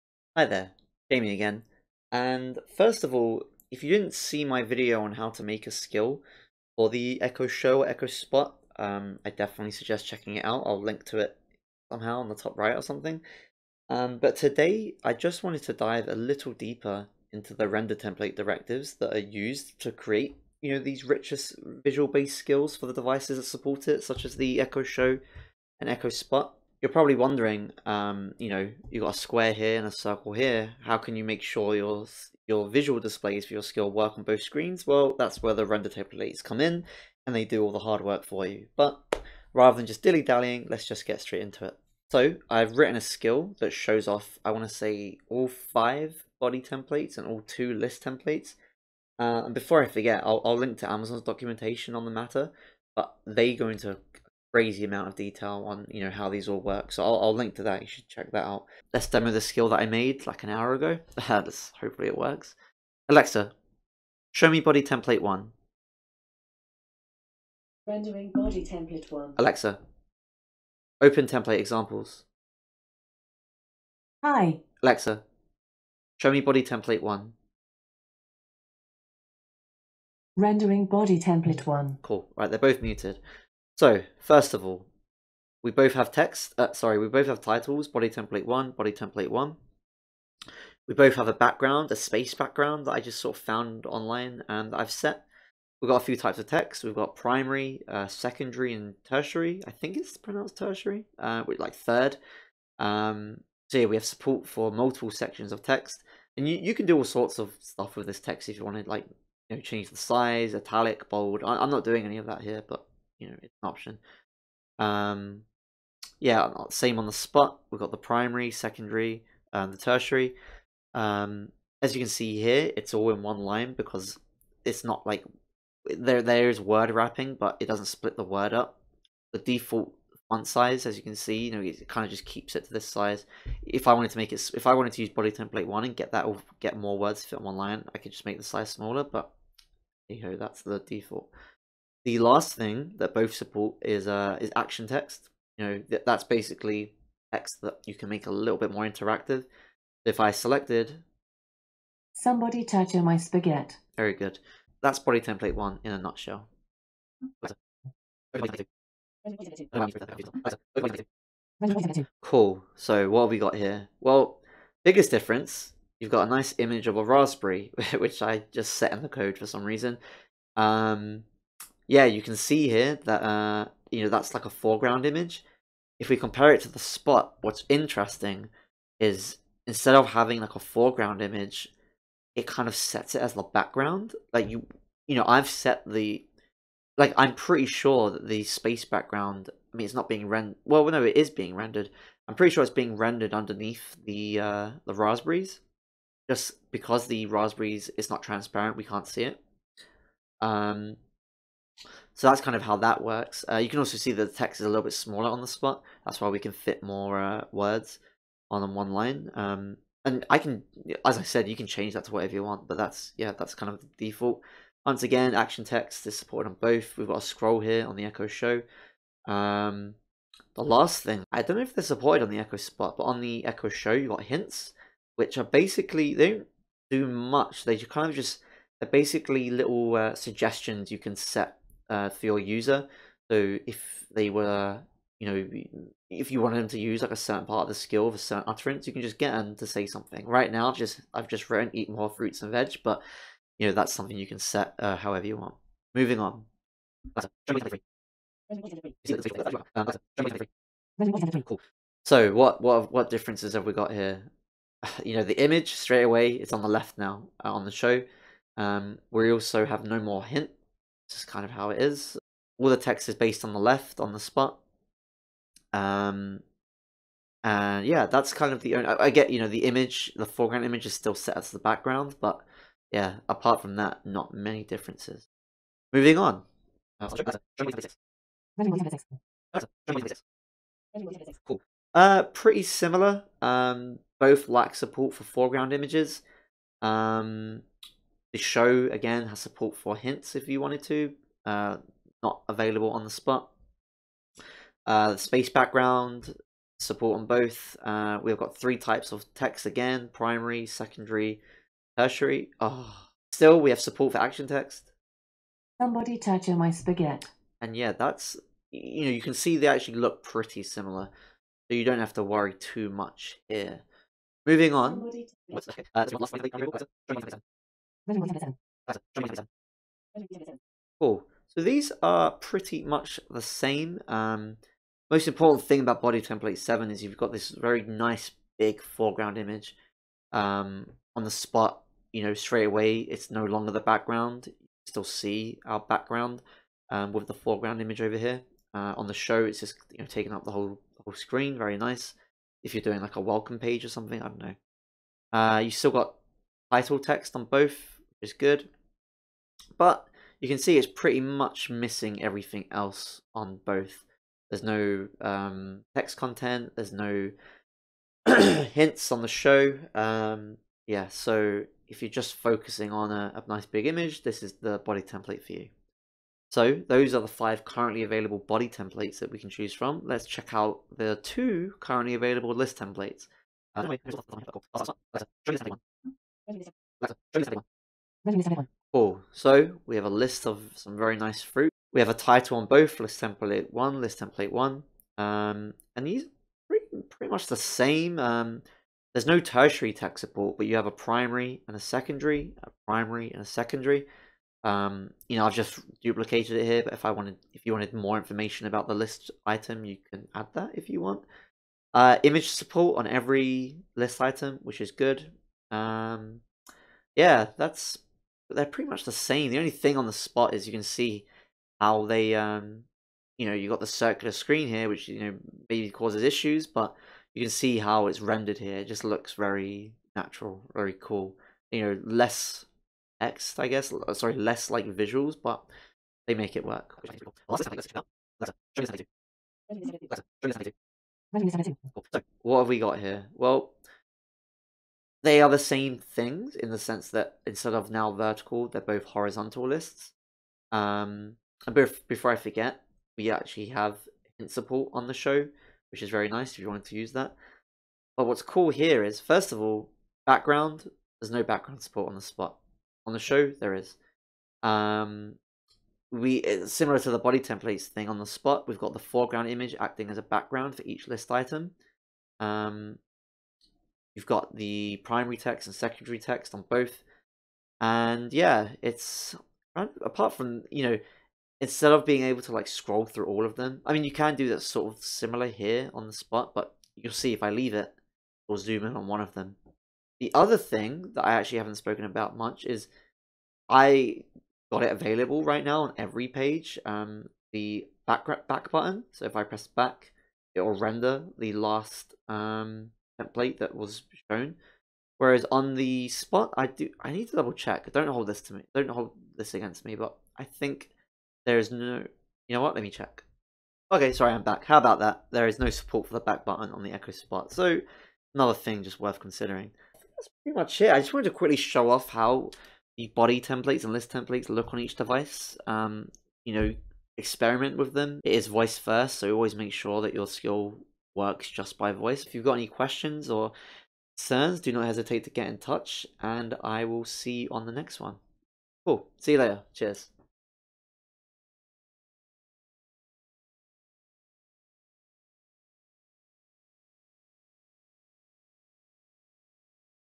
Exploring how Body Templates/List Templates Render on the Echo Show/Echo Spot
Wanna check out the render template directives in a little more detail and see how they look on both the Echo Show/Echo Spot? You've come to the right place!

Questions: @jamielliottg
Render Template Docs